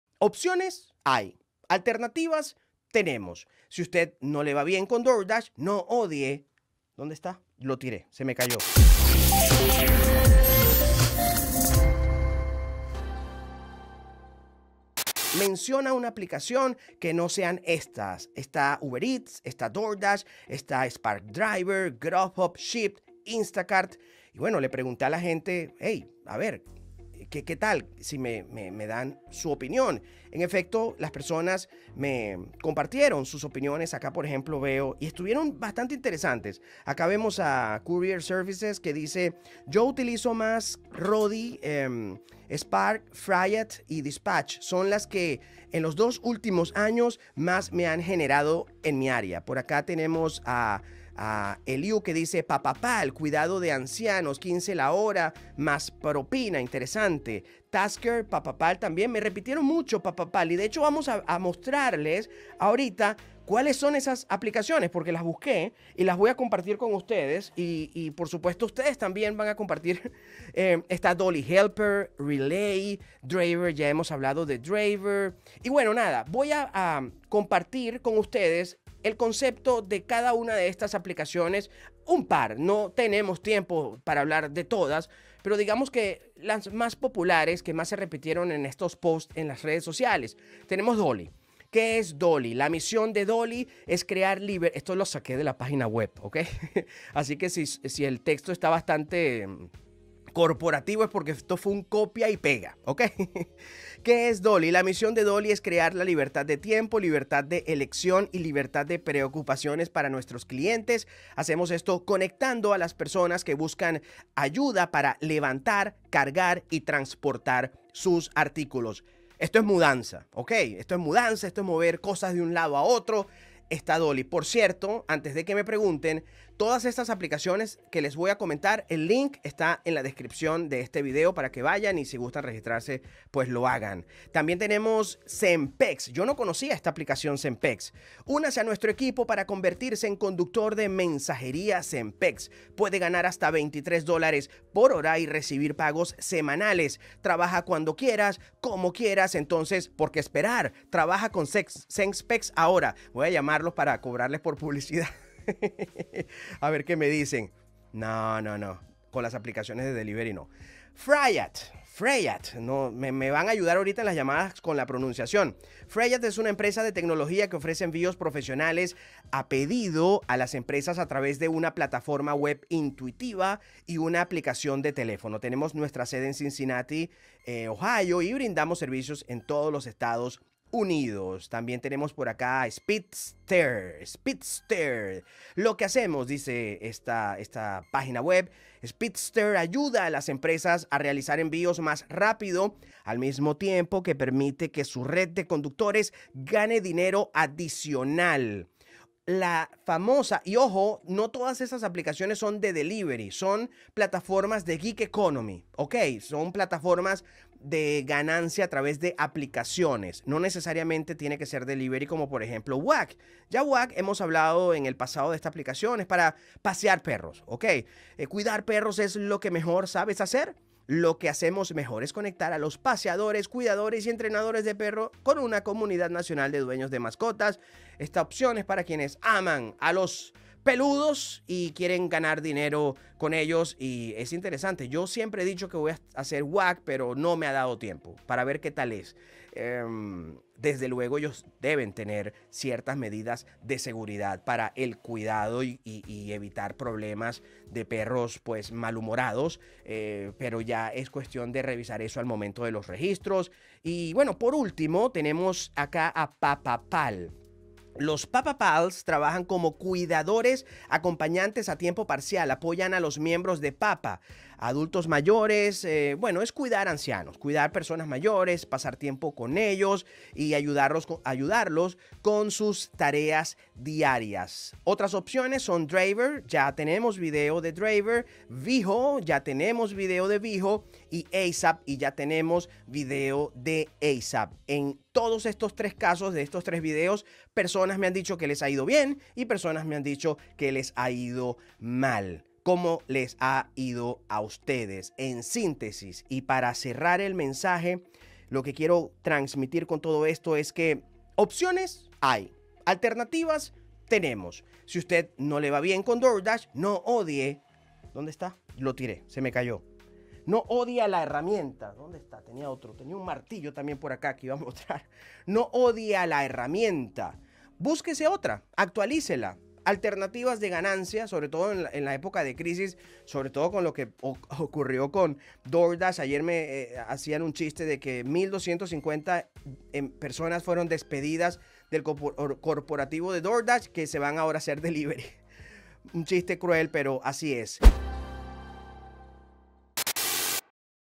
MENCIONA UNA APP Que No Sea Doordash O Uber - Evaluando Oportunidades De Ingresos
Existen muchas alternativas en el gigeconomy, estudiemos un par y no dejen de compartirnos nuevas opciones.